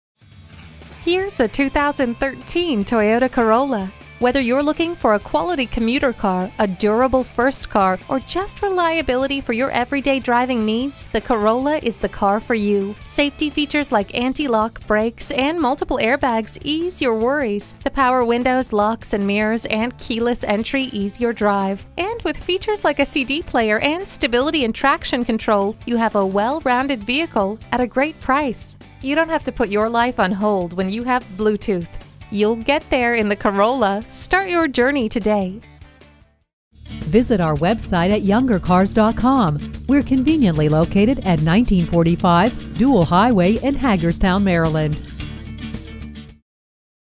Certified 2013 Toyota Corolla Hagerstown, WV V2289102 - SOLD
Year: 2013
Make: Toyota
Model: Corolla
Trim: S Special Edition
Engine: 4 Cyl. 1.8L
Transmission: Automatic
Color: Hot Lava
Interior: Black
Mileage: 18034
Stock #: V2289102
VIN: 2T1BU4EE7DC084984
IIHS Top Safety Pick. Only 18,034 Miles! Boasts 34 Highway MPG and 26 City MPG! Carfax One-Owner Vehicle. Dealer Certified Pre-Owned. This Toyota Corolla delivers a Gas I4 1.8L/110 engine powering this Automatic transmission. Torsion beam rear suspension, Tire pressure monitor system, Temporary spare tire.*This Toyota Corolla Features the Following Options *Star safety system -inc: enhanced vehicle stability control (VSC), traction control (TRAC), anti-lock brake system (ABS), electronic brake-force distribution (EBD), brake assist (BA), Smart Stop Technology (SST), Sport instrumentation -inc: tachometer, twin tripmeters, Special edition floor mats, Side-impact door beams, Rocker moldings, Remote keyless entry -inc: panic feature, remote illumination, trunk-release, Rear window defogger w/timer, Pwr windows w/driver one-touch down, P215/45R17 all-season tires, Multi-reflector halogen headlamps w/black sport trim.*This Toyota Corolla is a Superstar! *IIHS Top Safety Pick, KBB.com Best Resale Value Awards, KBB.com Brand Image Awards.*Stop By Today *A short visit to Younger Toyota Motorcars located at 1935 Dual Highway, Hagerstown, MD 21740 can get you a hassle-free deal on this tried-and-true Corolla today!

Be One!